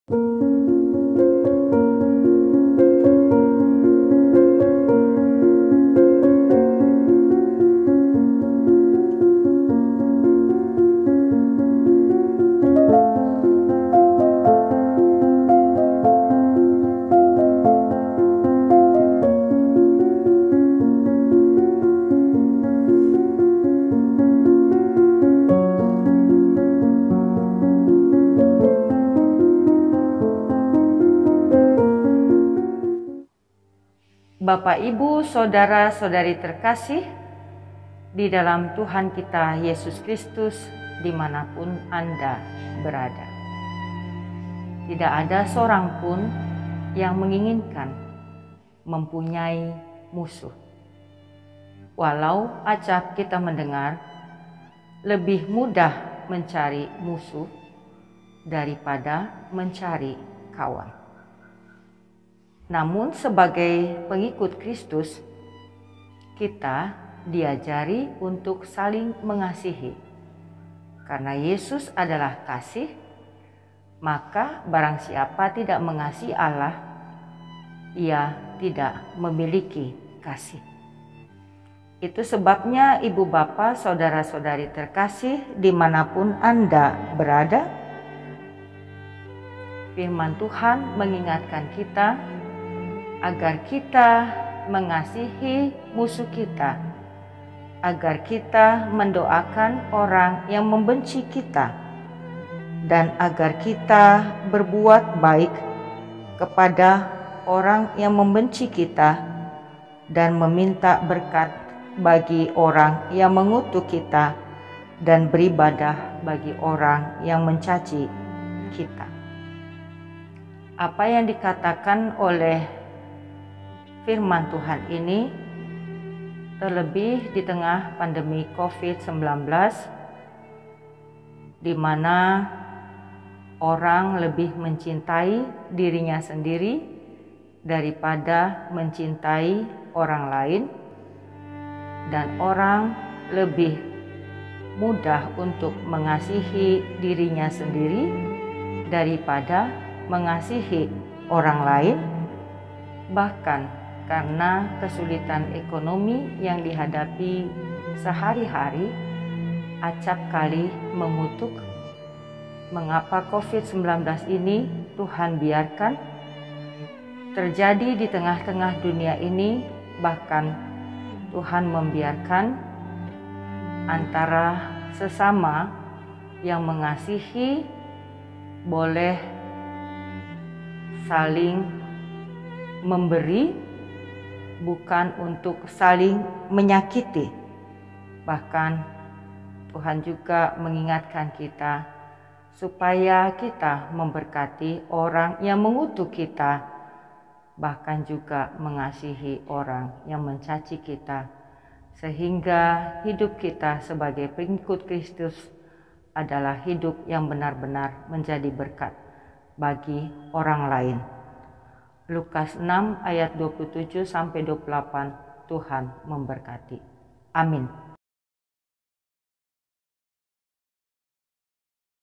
Renungan Jumat 26 Feb 2021Oleh KEPALA DEPARTEMEN DIAKONIA HKBP Pdt Debora Purada Sinaga,M.Th
Lukas 6:27-28 (TB)  "Tetapi kepada kamu, yang mendengarkan Aku, Aku berkata: Kasihilah musuhmu, berbuatlah baik kepada orang yang membenci kamu;
mintalah berkat bagi orang yang mengutuk kamu; berdoalah bagi orang yang mencaci kamu.

Luke 6:27-28 (KJV)  But I say unto you which hear, Love your enemies, do good to them which hate you,
Bless them that curse you, and pray for them which despitefully use you.